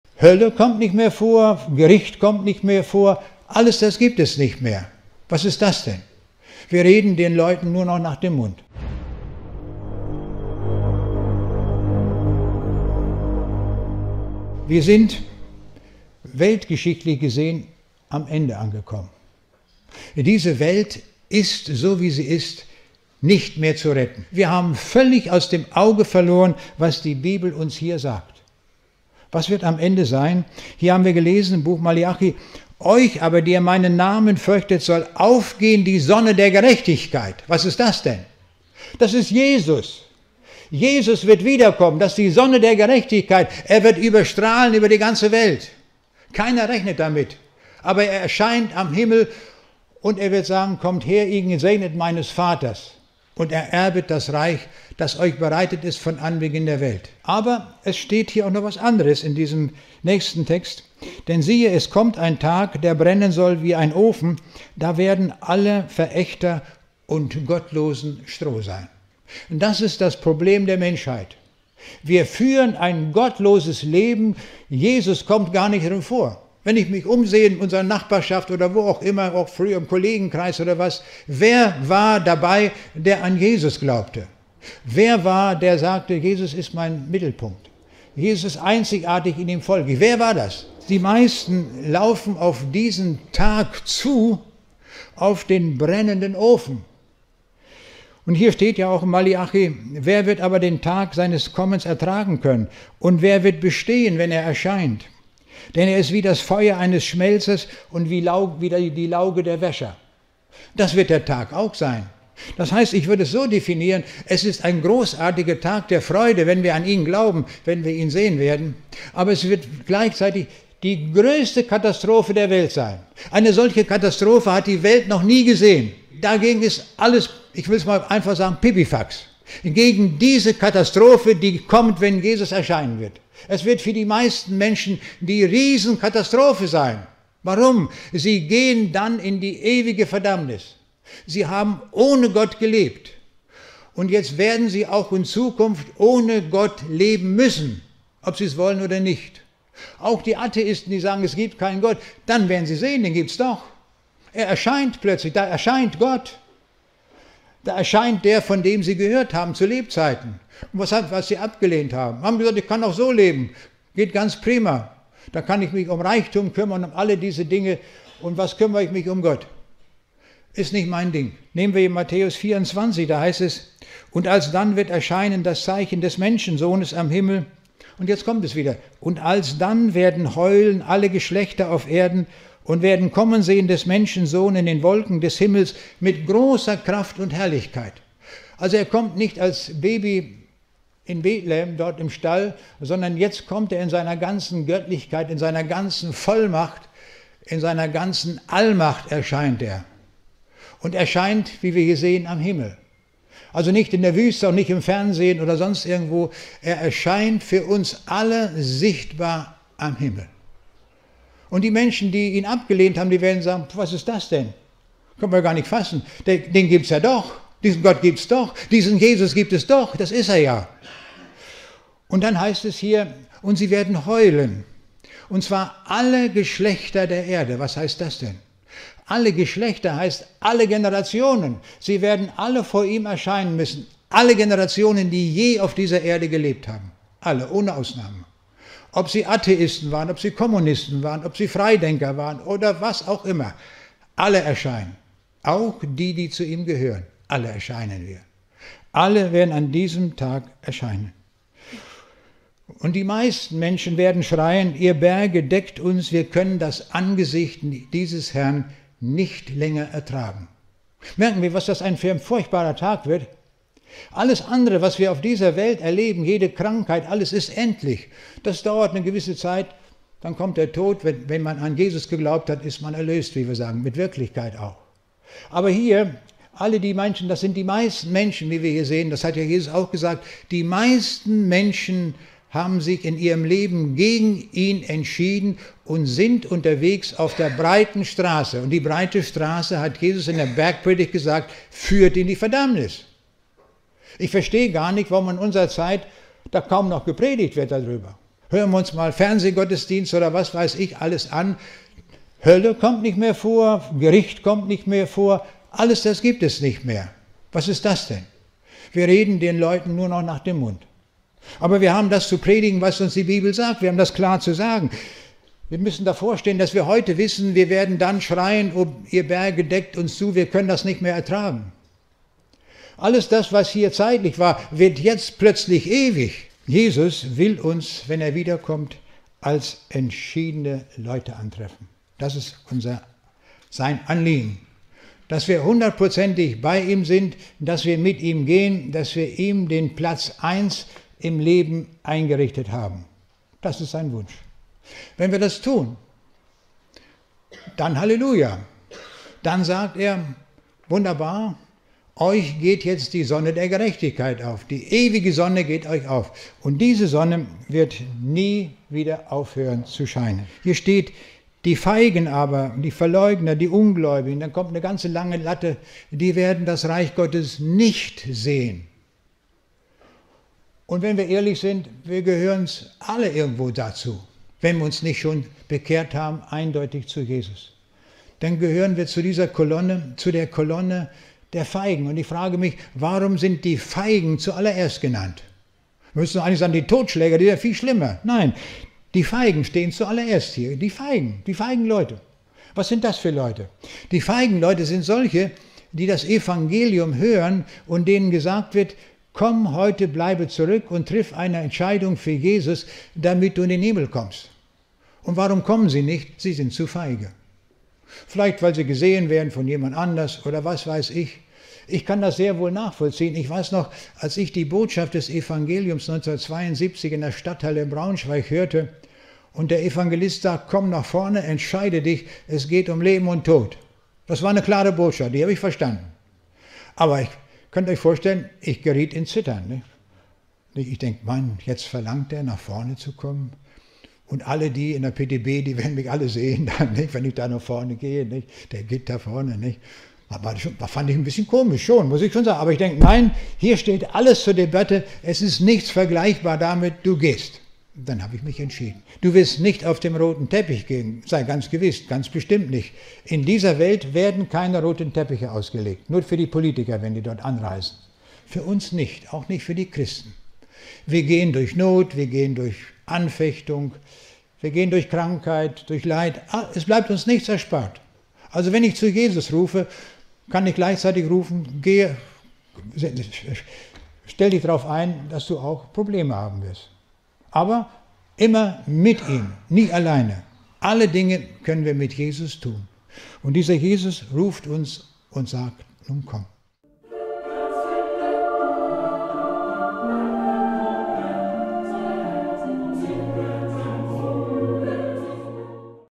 Dr. Werner Gitt kritisiert die weichen Christen! Hölle und Gericht werden nicht mehr genannt.
Hier erfolgt eine deutliche Warnung von Dr. Werner Gitt. Jeder wird vor dem Richterstuhl Gottes stehen und die Hölle existiert. Die weiche links-grüne Kirche möchte nur noch eine wohl fühl Atmosphäre schaffen, aber Gottes Wort wird oft vernachlässigt. Prediger wie Pastor Olaf Latzel oder Dr. Werner Gitt braucht die weichgespülte Kirche. Corona, Inflation, Krieg mit Russland usw. sind nichts im Vergleich mit Gottes Gericht in der Endzeit.

Mein lieber Freund wir leben in der Endzeit und Jesus Christus wird bald wieder kommen.

Da Youtube meine Videos zensiert und meinen Kanal mit der Löschung bedroht, so schaue doch einfach mal vorbei